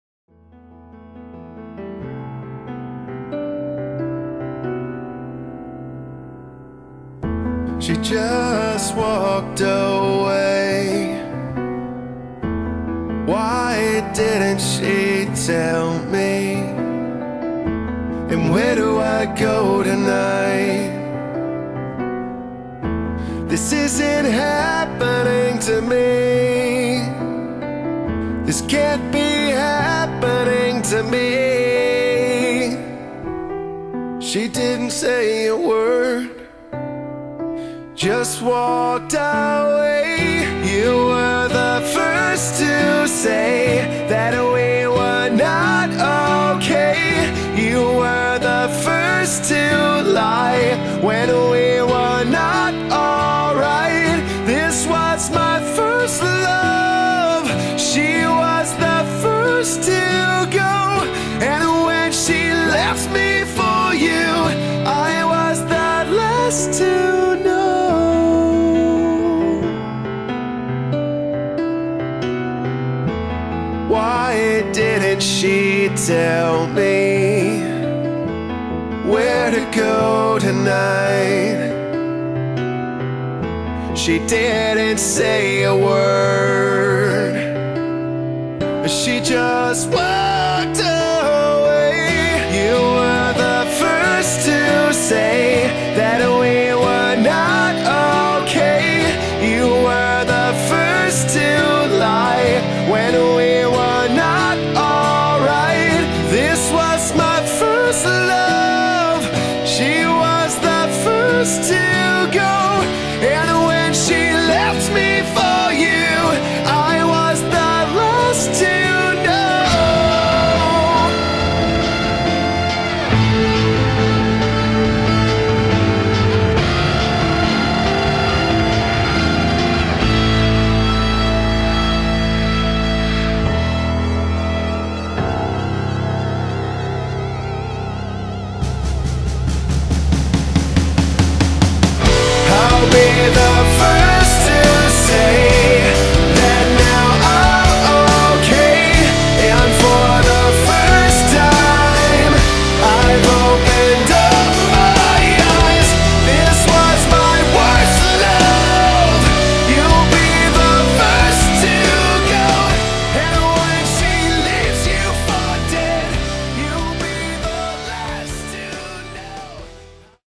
"LAST TO KNOW" by Christina Becker
Trailer del OS "Last to Know" en FanFiction Fifty Shades Trilogy
"—¿Tú qué crees que deberías hacer, Christian?"
"—No sé qué hacer, John. Una parte de mí piensa que lo mejor que pudo hacer Anastasia es marcharse, alejarse de mí. No soy el hombre que ella se merece." Christian nos explica cómo pasó esos cinco días sin Anastasia. Él está destrozado, pero quiere a Anastasia a su lado cueste lo que cueste. Está firmemente decidido en lo que hará.